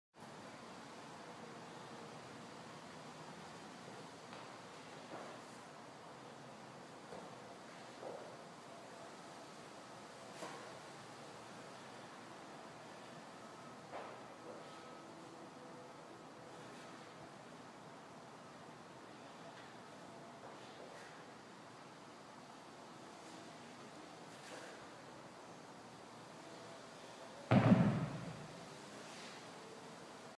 1/2 Getups
First portion of the full Getup; great warmup drill. Very good corrective exercise for improving glute function